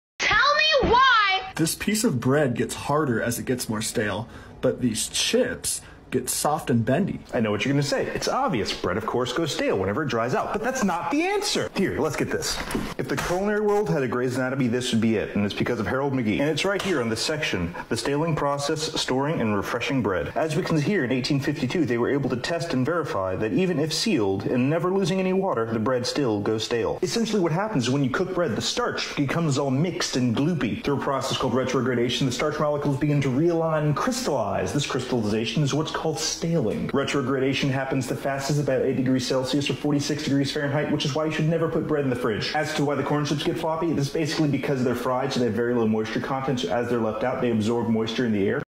Stale Bread Doesn't Mean Dry
Stale bread isn't inherently dry. It's a chemical process that can be reversed by heating.